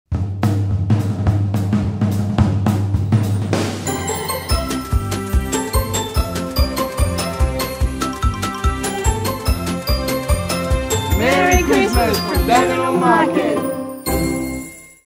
Christmas at Dandenong Market - A Food Haven!
Take a glimpse into Christmas @DandenongMarket where all the flavours of the world are here to delight this festive season.